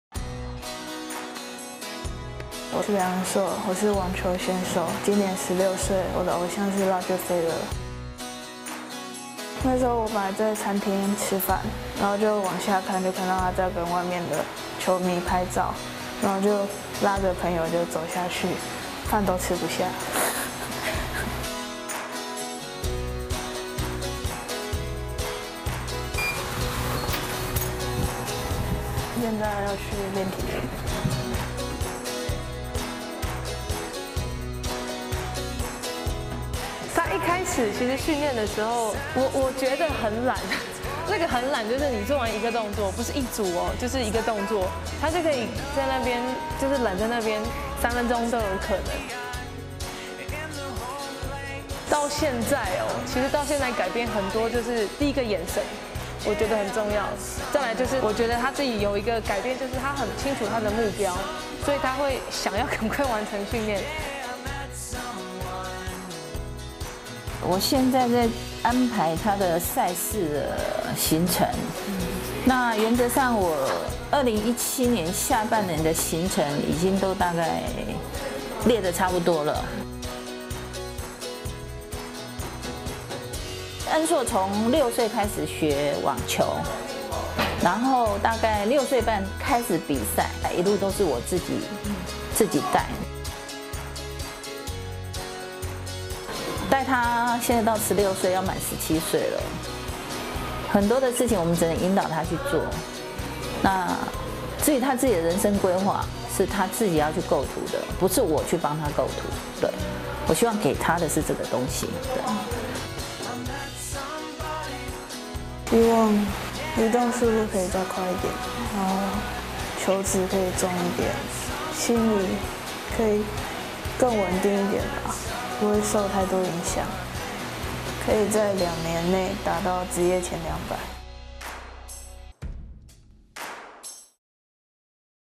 邁向巔峰之路》網球新星梁恩碩 目標闖進職業前200
梁恩碩，從六歲開始練網球，六歲半開始比賽，ITF青少女最高世界排名17，媽媽更是全職陪伴女兒四處征戰。

14日在ITF賽事泰國站女子雙打決賽中以直落二封后，拿下第一個ITF職業賽的冠軍！隔日則在女單決賽中力拼台北世大運銅牌得主，雖然最後敗下陣來，拿下亞軍，不過仍然是值得期待的網球新星！

現在就一起來看看她平常的訓練生活吧！